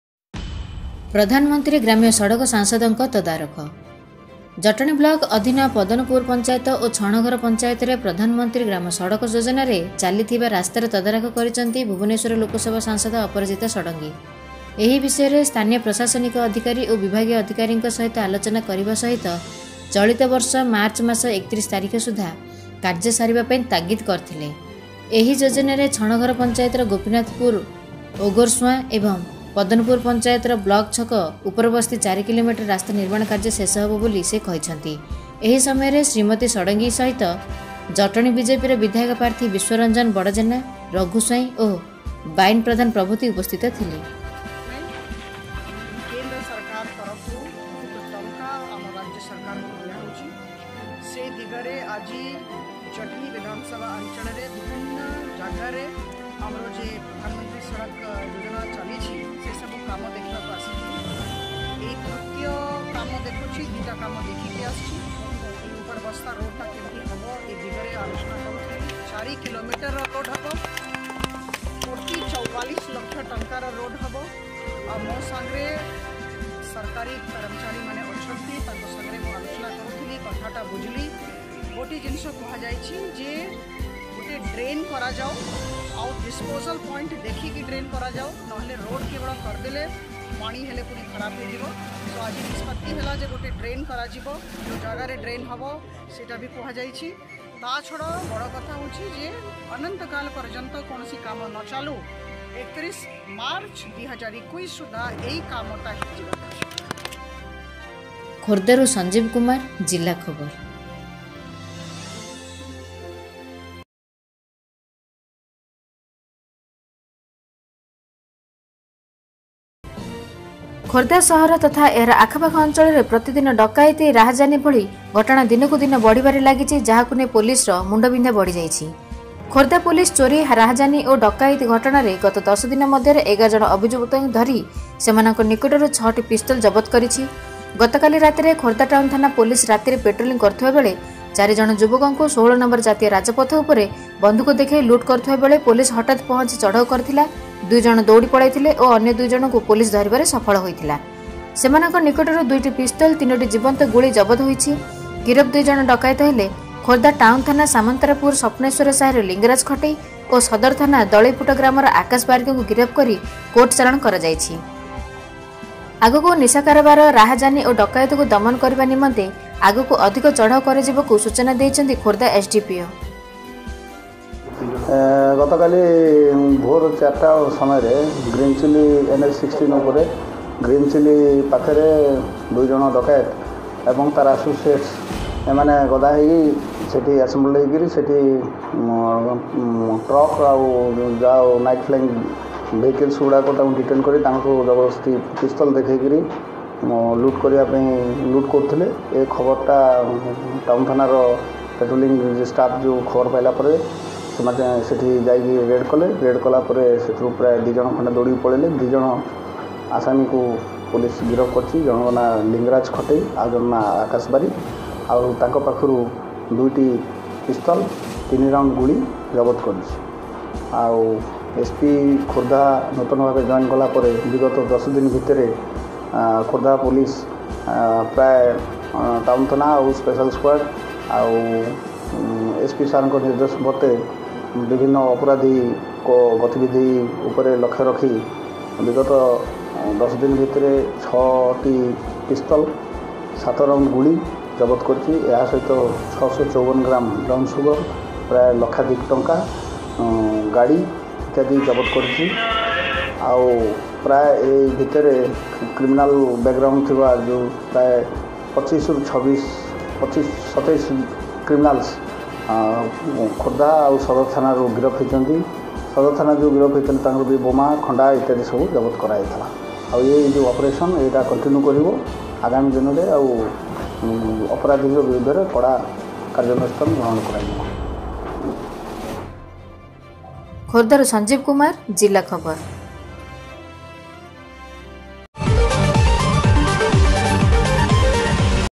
@Zillakhabarnews#Zillakhabar.com#
*ପ୍ରଧାନମନ୍ତ୍ରୀ ଗ୍ରାମ୍ୟ ସଡକ ସାଂସଦ ଙ୍କ ତଦାରଖ
*ଡକାୟତି ଉଦ୍ୟମ ଦୁଇ ଗିରଫ ୨ ପିସ୍ତଲ ଜବତ